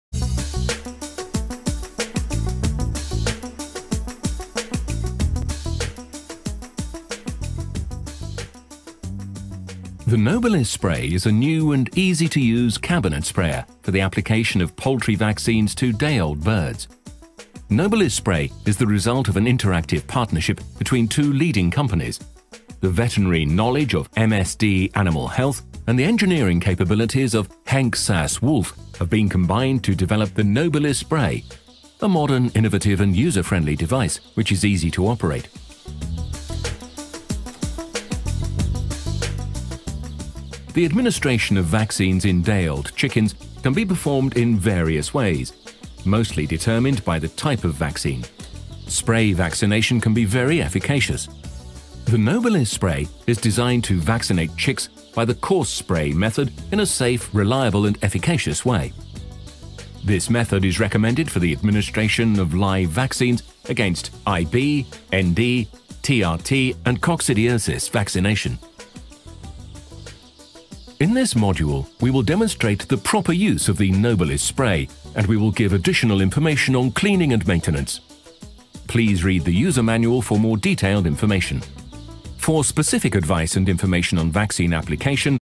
MSD Nobilis Spray Device
Easy and efficacious spray device for day old chick vaccination.